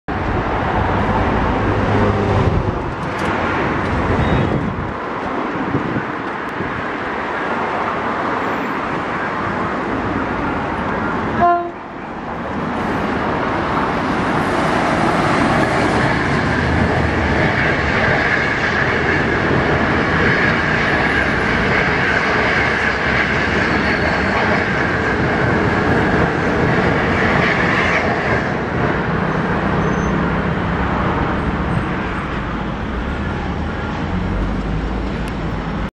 [HD] The Northbound passenger train no. T102 (HXD1D 0224 + 25K) at ZhangMuTou
The Northbound passenger train no. T102 (ShenZhen to ShangHai Nan {South}) haul by HXD1D 0224 + 25K pass the ZhangMuTou.

Date: 30/12/2018
Canon G3X 1920 × 1080 25P FHD
Place: LiYuan Road, ZhangMuTou